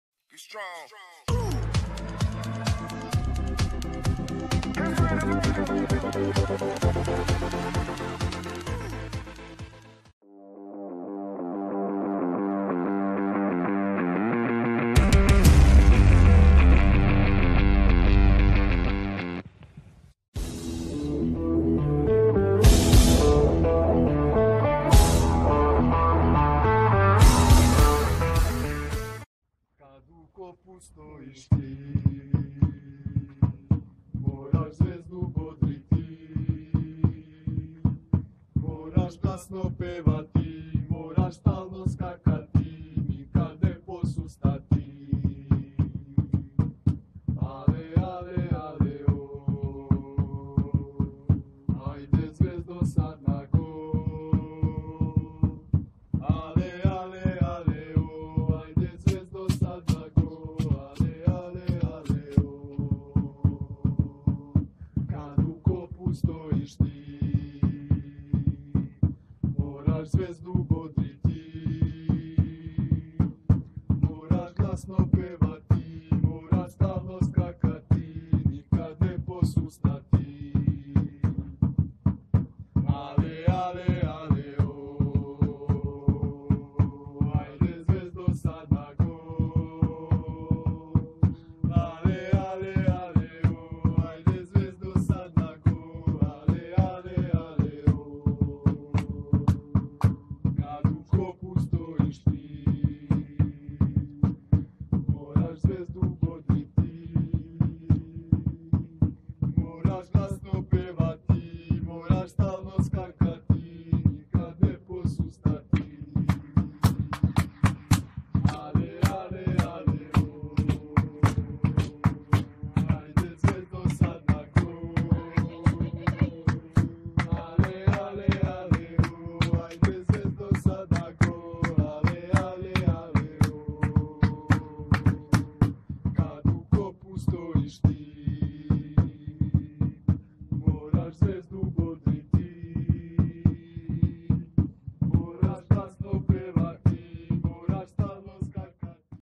Kaleb Tarczewski | WELCOME TO CRVENA ZVEZDA? | HD
Ime koje se sve češće provlači kao potencijalno pojačanje jeste Kaleb Tarzuski. Amerikanac već pet godina nastupa za italijanski Milano. Njegov ugovor ističe tek naredno leto, ali italijanski mediji već duže vreme pišu o njegovom rastanku sa ekipom koju predvodi Etore Mesina. Priča o Tarzuskom nije novost, s obzirom da je Dejan Radonjić bacio oko na centra i prošle godine, ali tada nije došlo do dogovora.

Kaleb Tarczewski statistics in video!